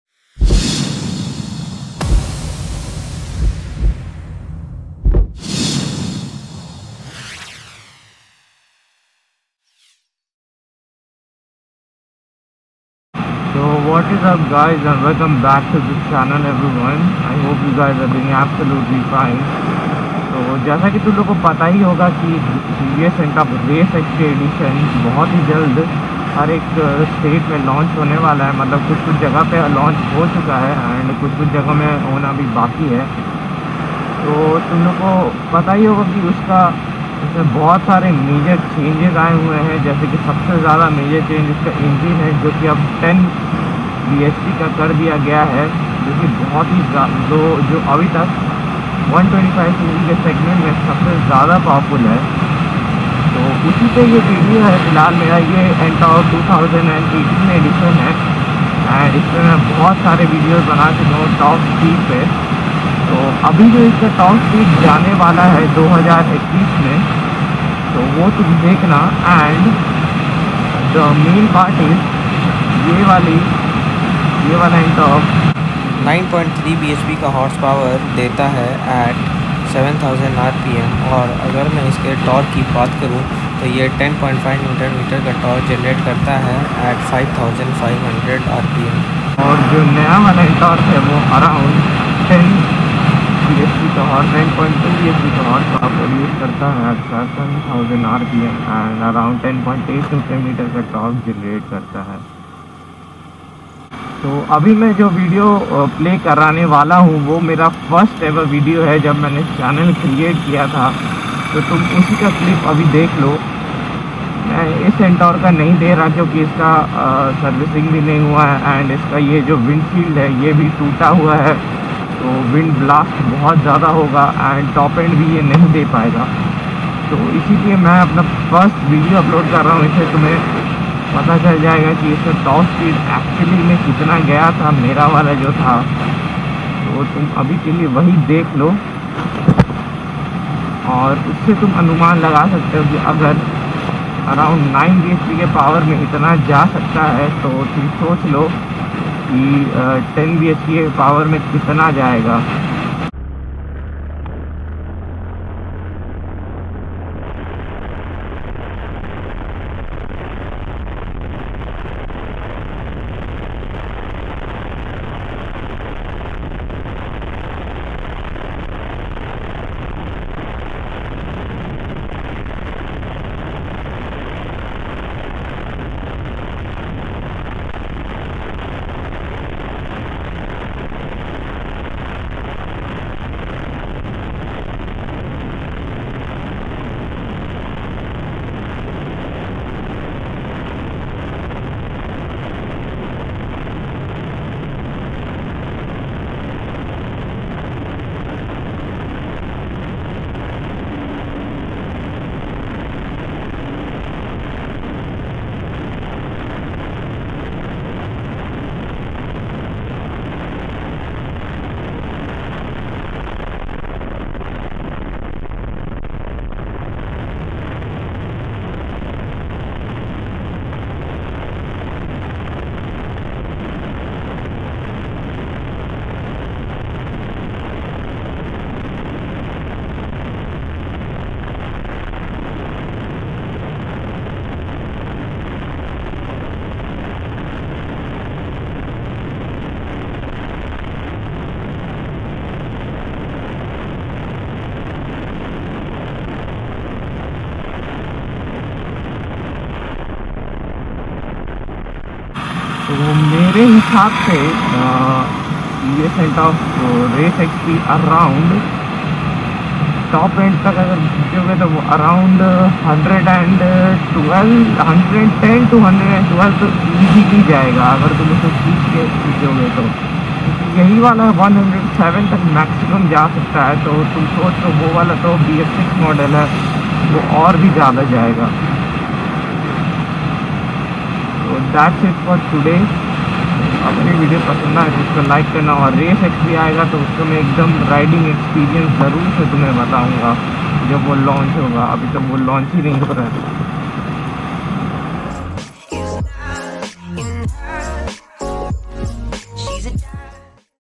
Tvs Ntorq 125 Race Xp Top Speed | Fastest 125cc Scooter?? | 2021
hey guys and welcome back to the channel everyone I hope you guys are doing well. So this video is basically about the ASSUMPTION WHAT COULD BE THE TOP SPEED OF UPCOMING TVS NTORQ 125 RACE XP WITH COMPARISON TO STANDARD EDITION. Since TV's ntorq race xp has not yet arrived at many states so it's not officially TV's ntorq race xp top speed just an assumption what could be the top speed as its the best scooty in 125cc category with 10bhp of power  and 10.8nm of torque. if you like this video then do subscribe the channel.